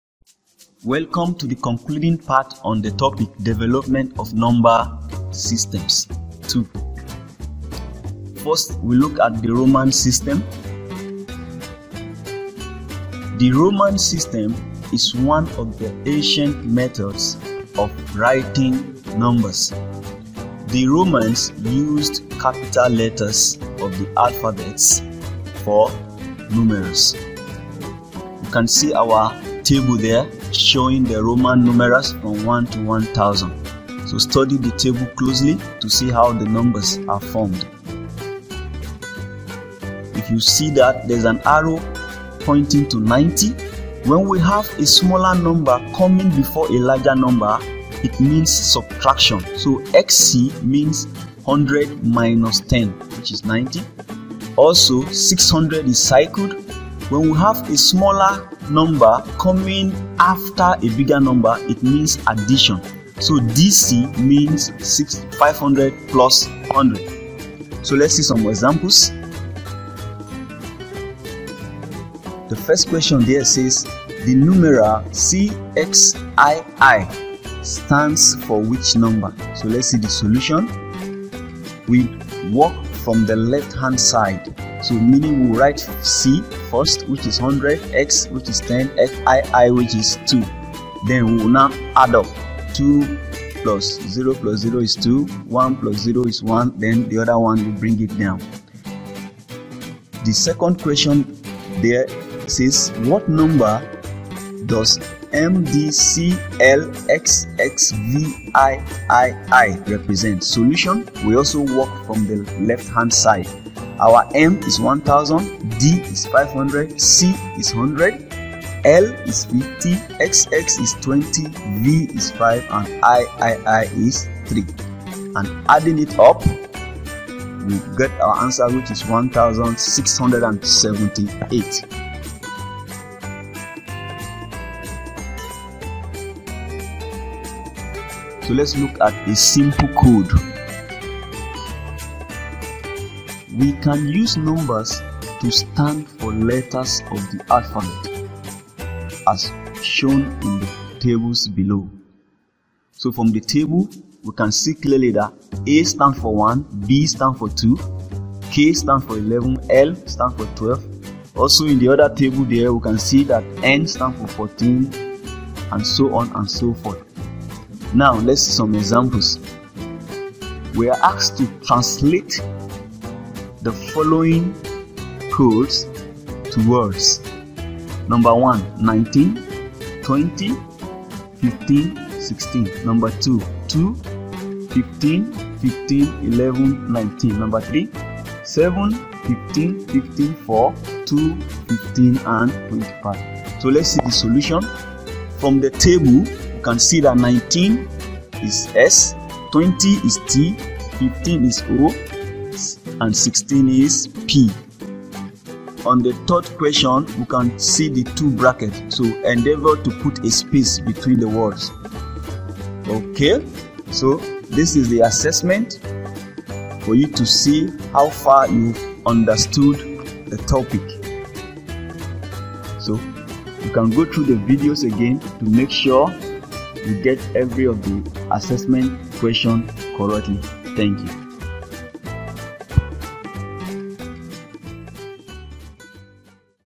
02 - Mathematics | J.S.S.1 | Development of Number System (Part 2)
In this Mathematics video we cover "Development of Number System Part 2" with specific highlights like the Roman System.
You can as well give us feedbacks to help us improve on the videos.